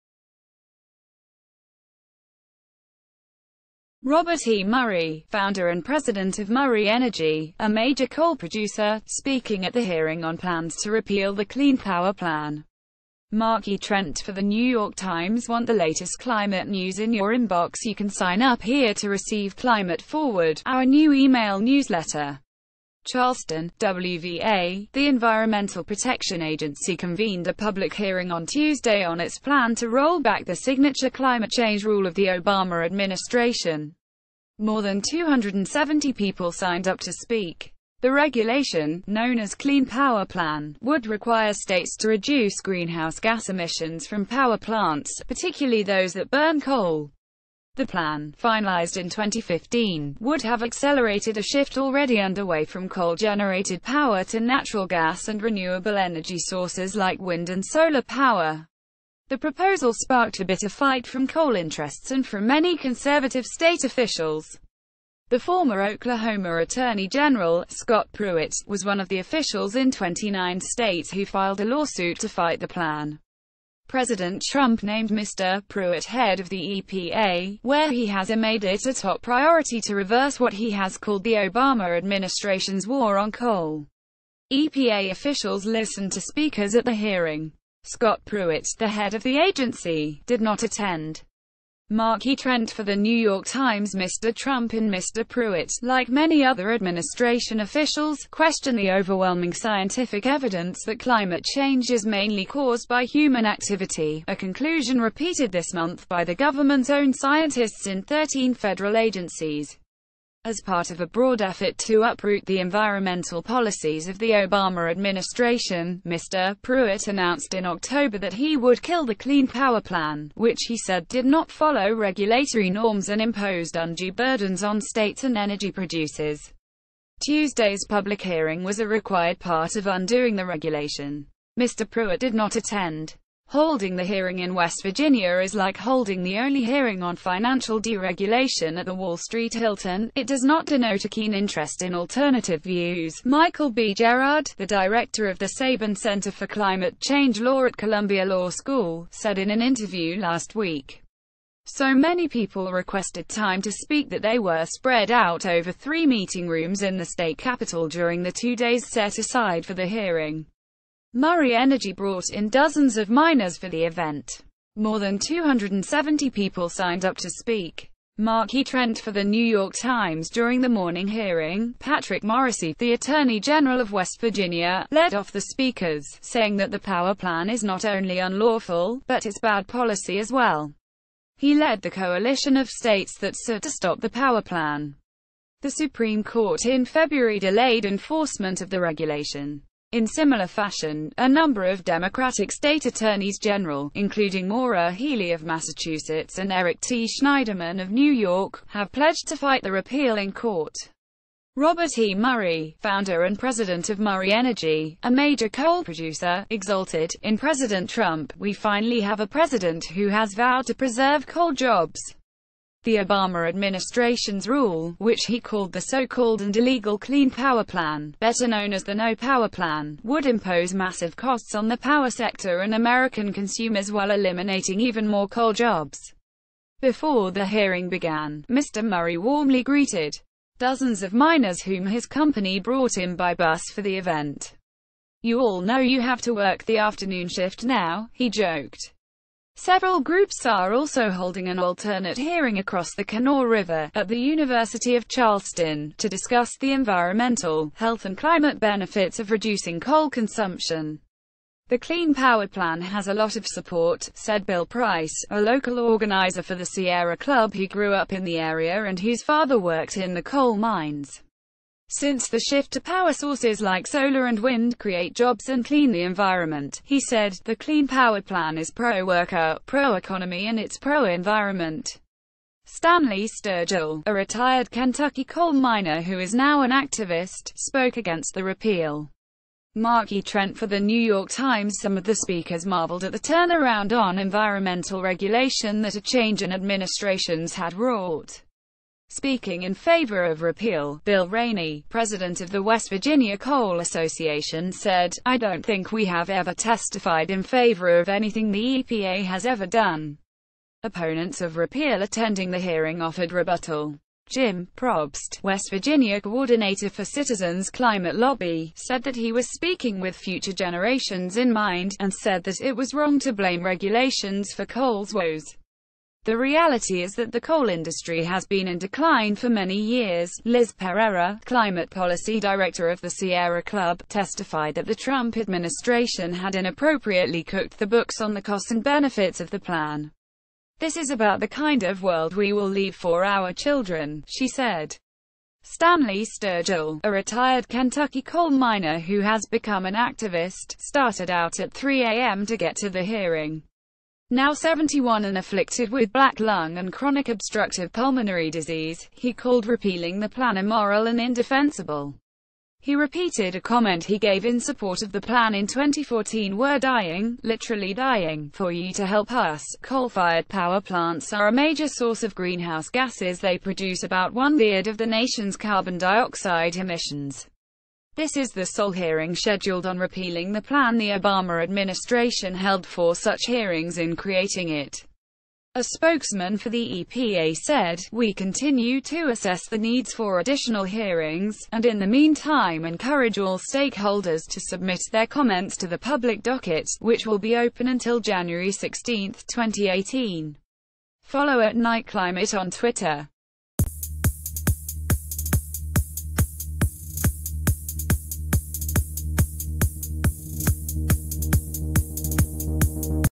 E.p.a. heads to coal country to hear views on an obama climate rule | News TV
Robert E. Murray, founder and president of Murray Energy, a major coal producer, speaking at the hearing on plans to repeal the Clean Power Plan. Mark E. Trent for The New York Times Want the latest c...